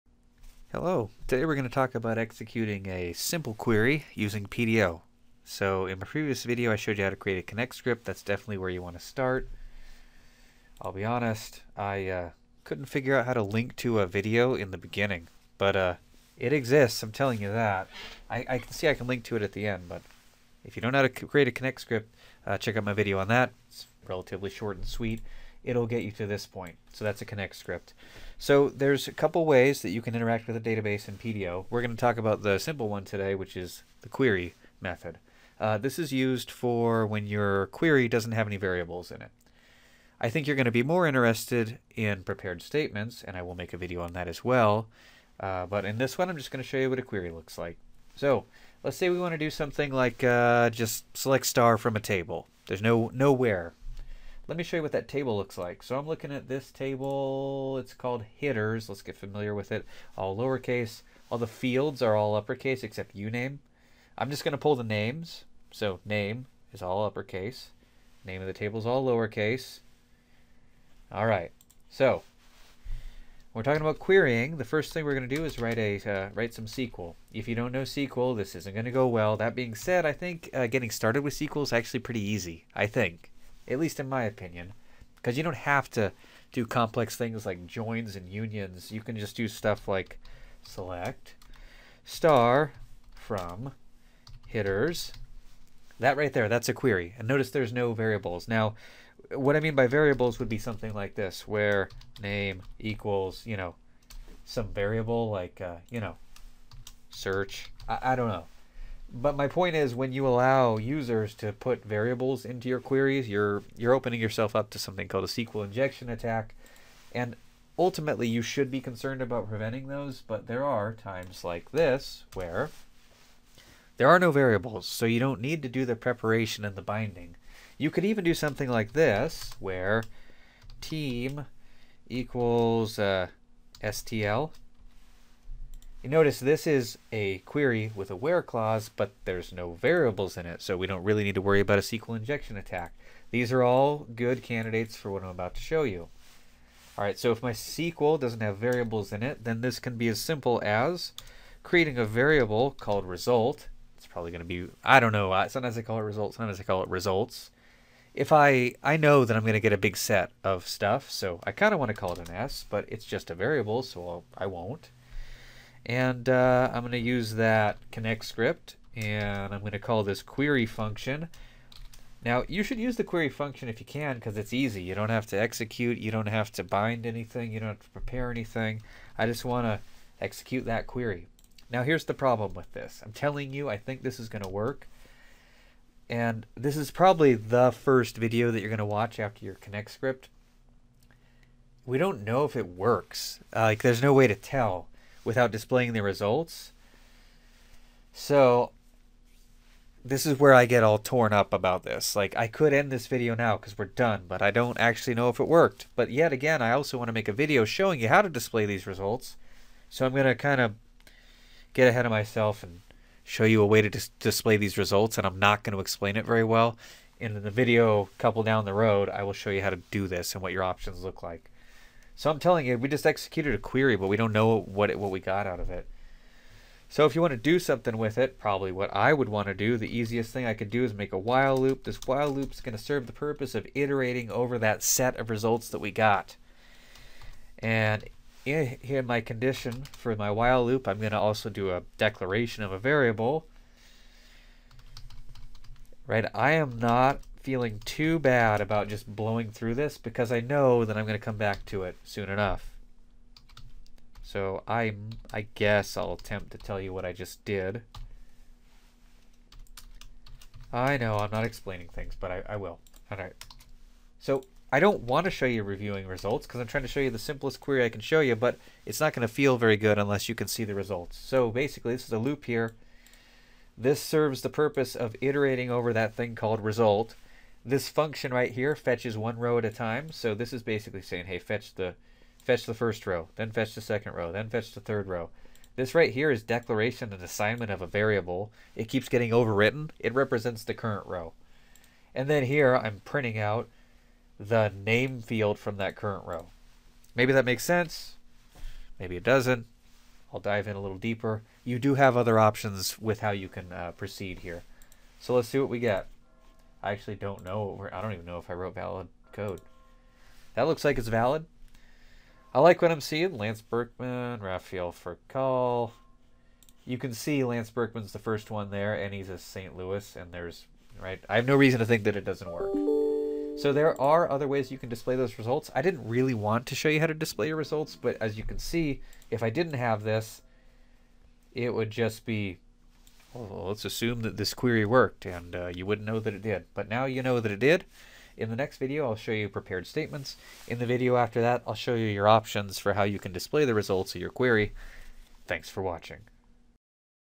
Execute a Database Query using PDO
In this video we will create and execute a simply database query using PDO. I will also briefly iterate over the results of the query.